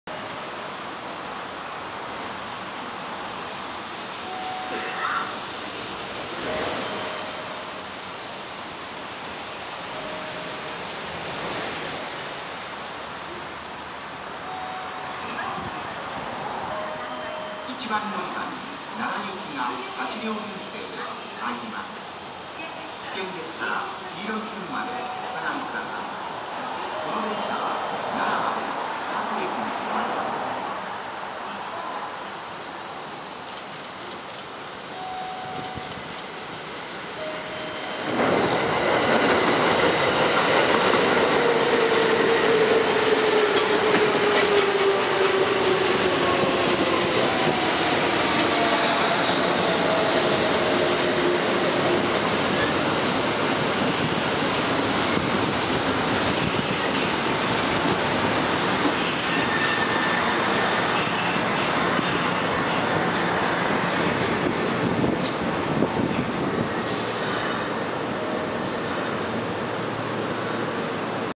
[近鉄電車] 奈良線 8400系L08編成+1233系VE37編成+8400系L04編成 快速急行｜大阪難波行き＠学園前
8400系L08編成+1233系VE37編成+8400系L04編成の10両編成です。